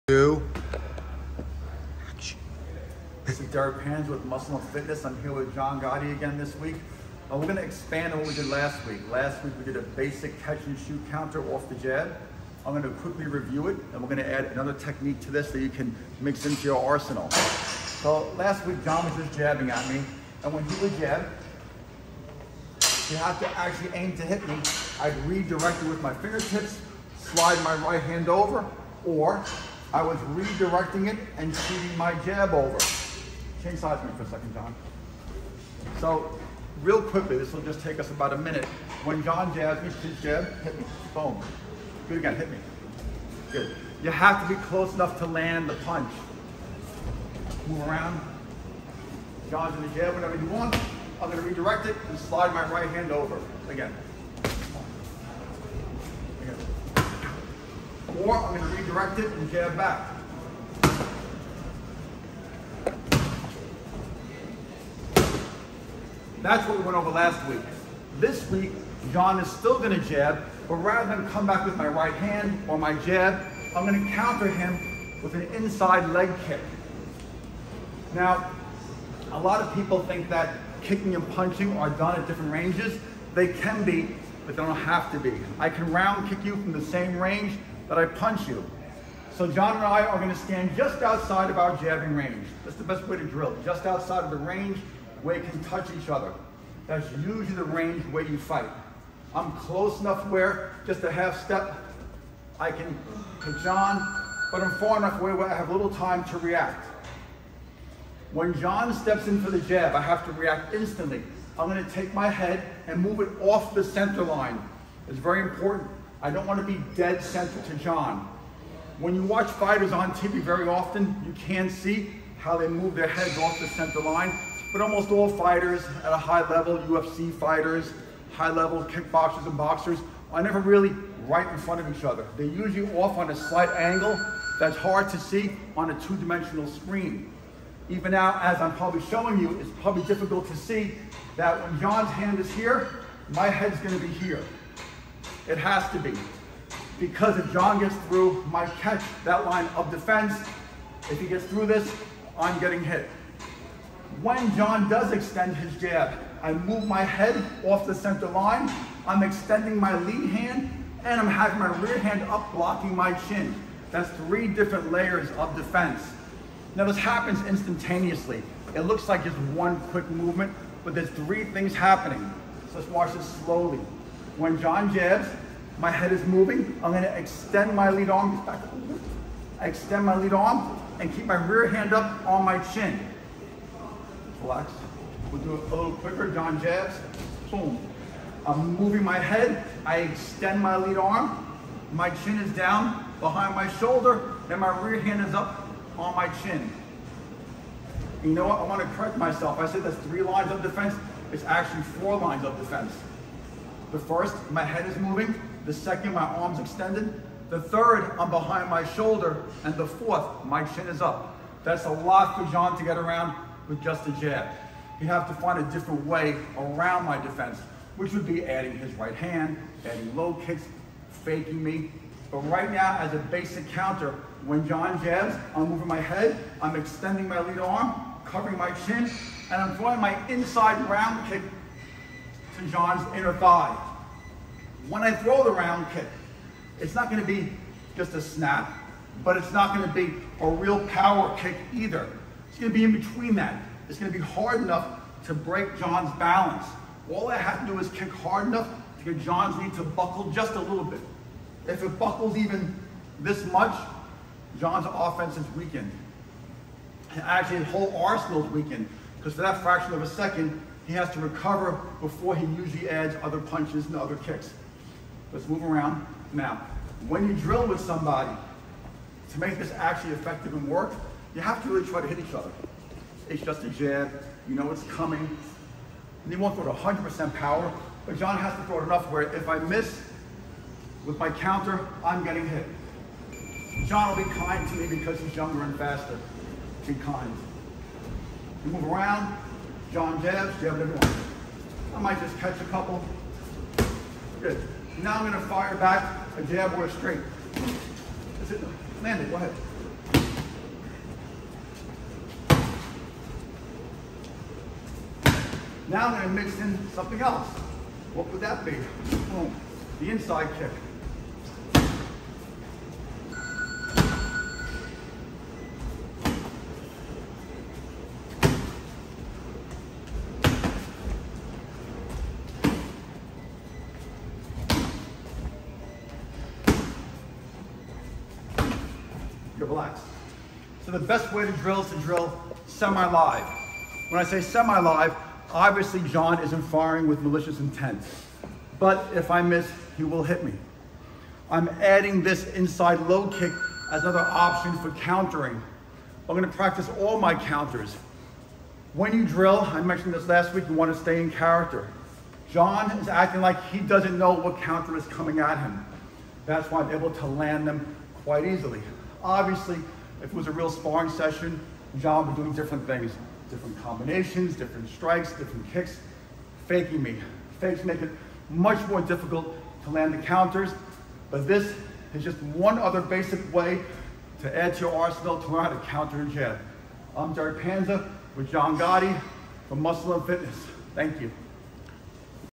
FIGHTING TECHNIQUES: Catch and Shoot Part II: Derek Panza & John Gotti III
FIGHTING TECHNIQUES: Catch and Shoot Part II:
Countering the jab with a Kick with 3x World Kickboxing Champion @derekpanza & fighter @jg3mma . Filmed at @bevsgym by @frank_sepe SEE FULL VIDEO ON @muscleandfitness YOU TUBE TONIGHT and next week @muscleandfitness